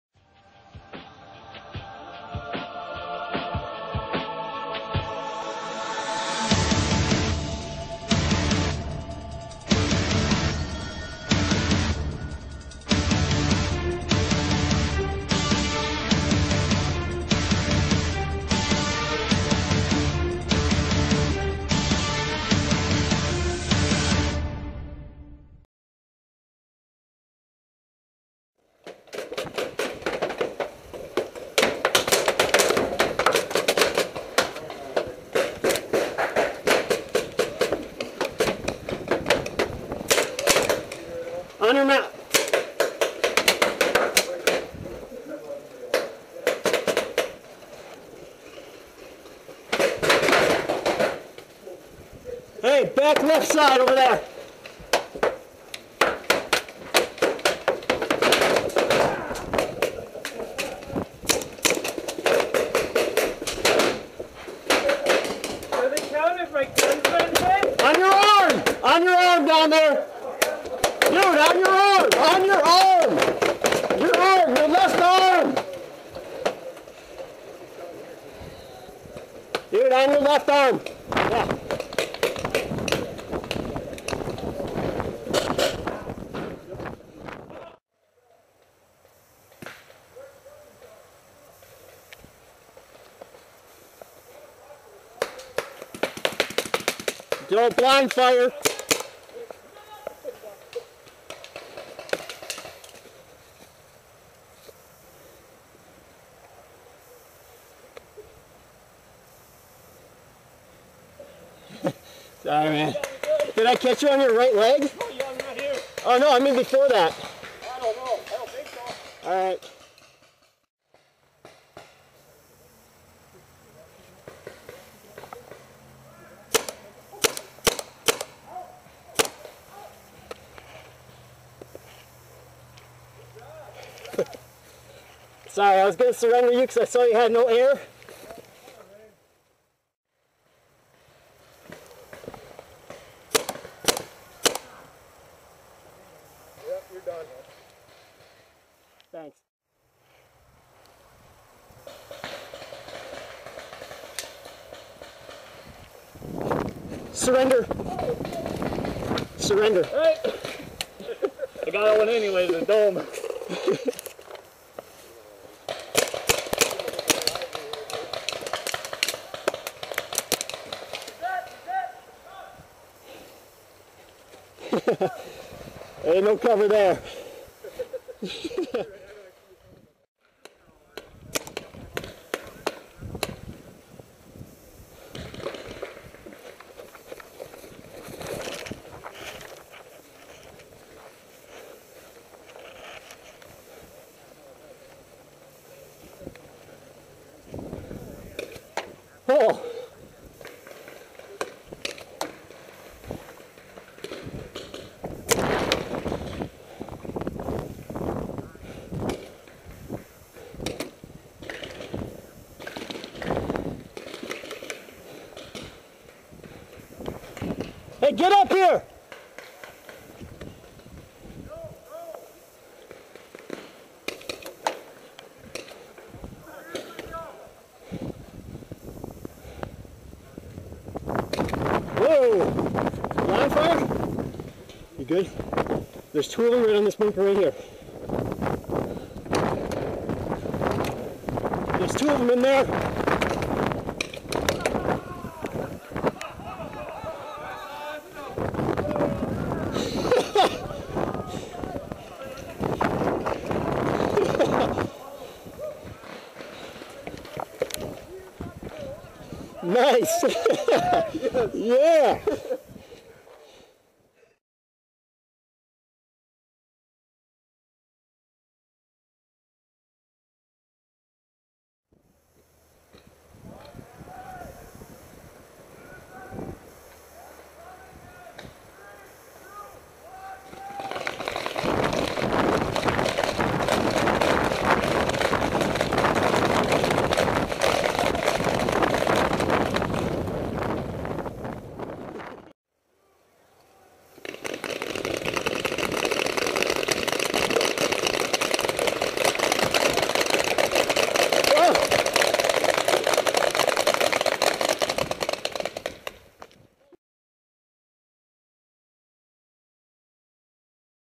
Highlights - Bachelor Party 2013
Some of the highlights from Justin Rust's bachelor party at Commando Paintball Sports in Abrams, WI.  They put our bachelor party together to go against another bachelor party.  At the end both Bachelors had to run the gauntlet.  Unfortunately it was a little dark and some of the footage was lost because of the darkness.  The darkness also took a little bit away from the final run too because the paint was a dark shell and couldn't be picked up too well.